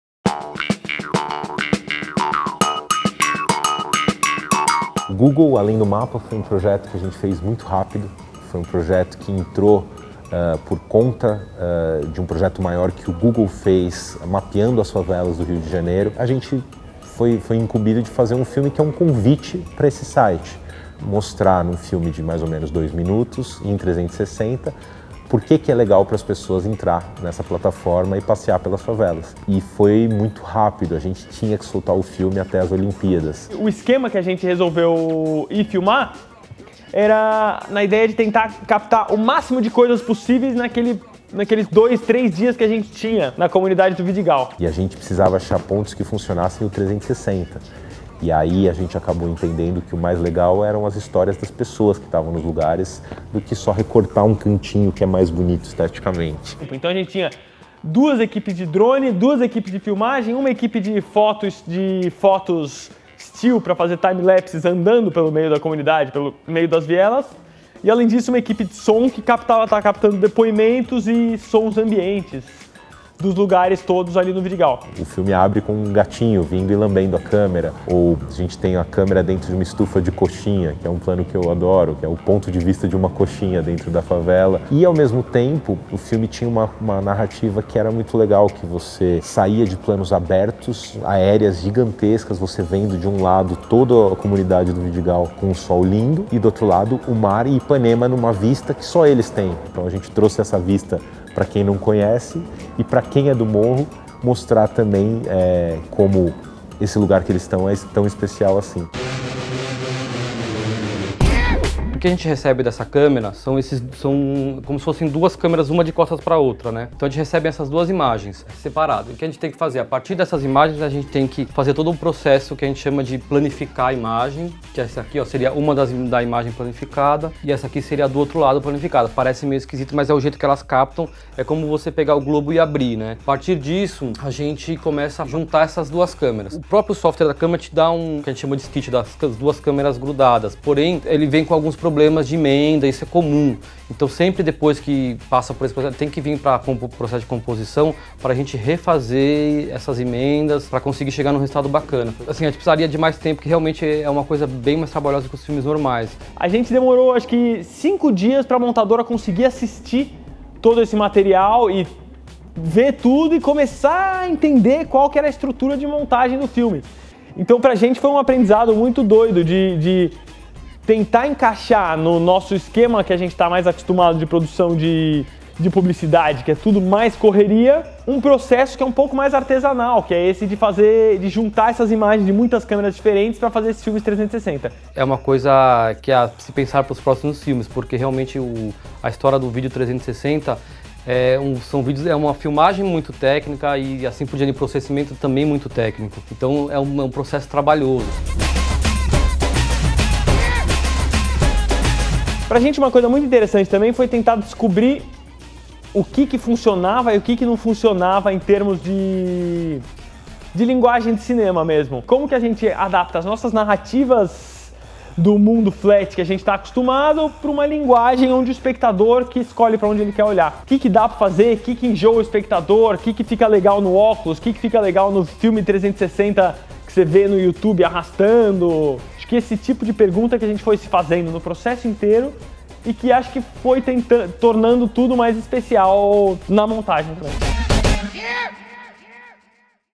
AQUI A GENTE FAZ ASSIM #58 - Dominando 360˚
Vídeo postado dia 25 de agosto de 2016 no site o2filmes.com

Os diretores Quico Meirelles e Ricardo Laganaro, responsáveis por um dos filmes do projeto Beyond the Map, do Google Arts and Culture, realizado em realidade virtual através do núcleo Outras Telas, se juntam a Martin Weisz, compositor da O2 Pós, e contam sobre o trabalho efetuado.